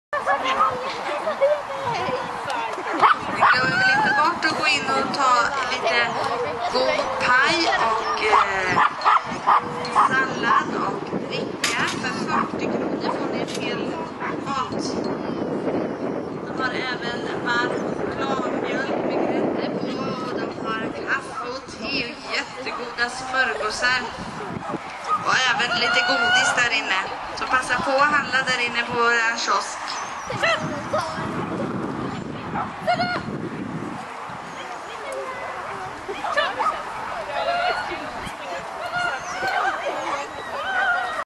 Agility Soderkoping
080501 Agility 3. Zorros debut som resulterade i förstaplats och cert.